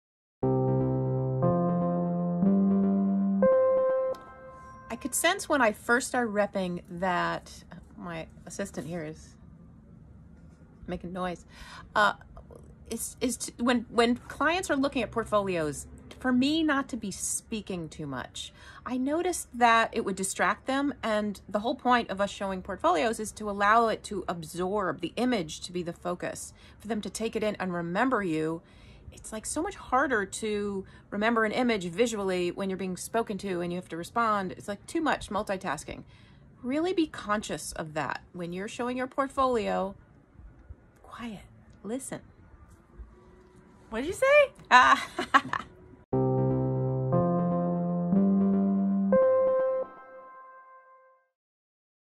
AskSternRep on Showing Your Portfolio
Commercial Photographer's Rep Andrea Stern of SternRep + AskSternRep discusses etiquette when showing your portfolio.